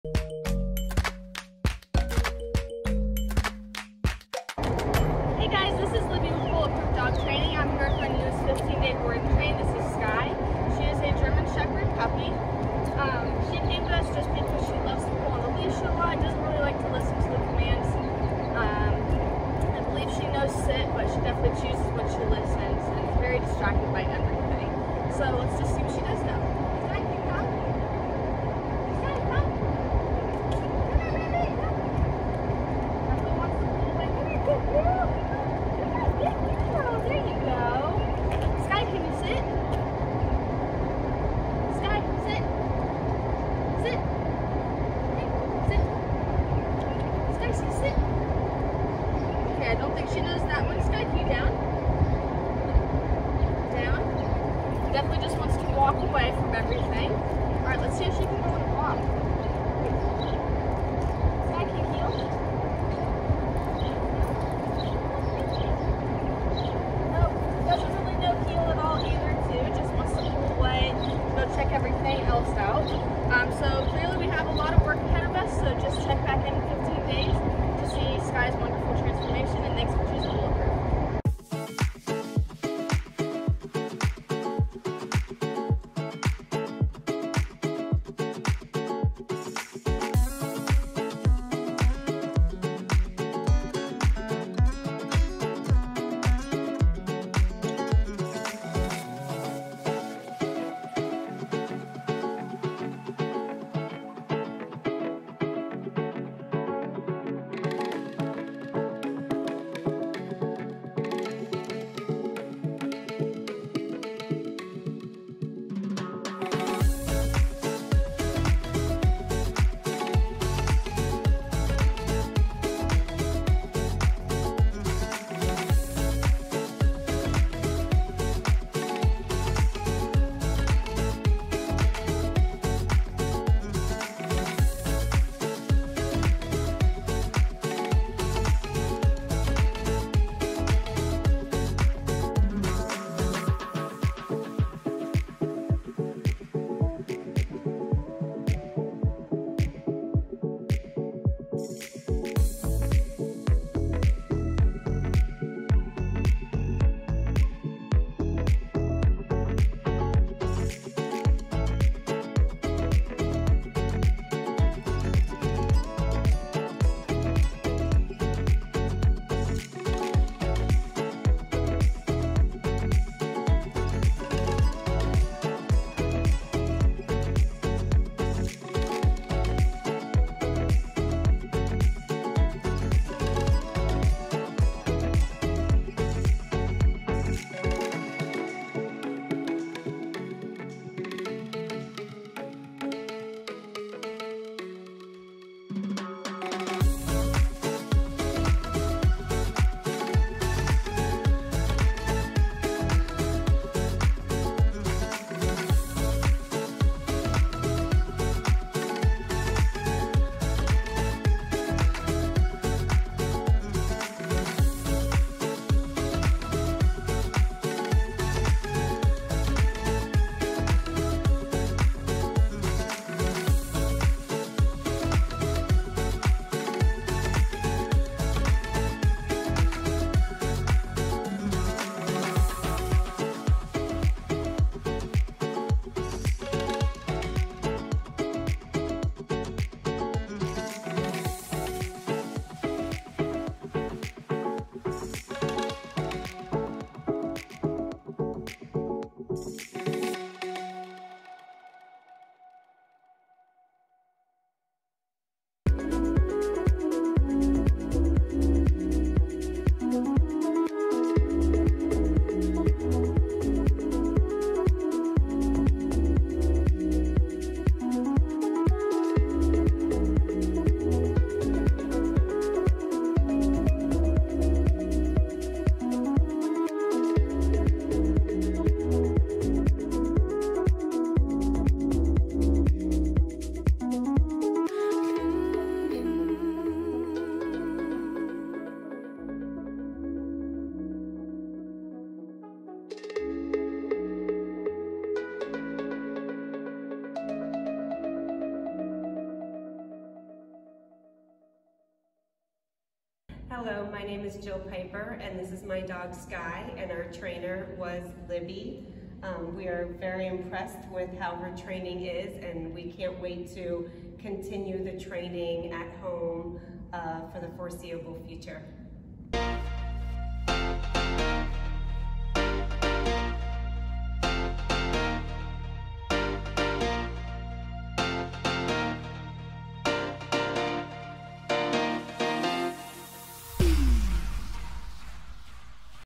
Amazing Transformation | "Skye" | German Shepard | bulletproofdogtraining | GSD
Timecodes:
Beginning - 0:00
Intro - 0:05
Heel - 1:21
Distance Down - 2:42
Place - 3:06
Duration Down - 3:23
Door Manners - 4:14
Load - 4:24
Reunion - 4:31
Owner Working Commands - 4:40
Testimonial - 5:03
Outro - 5:19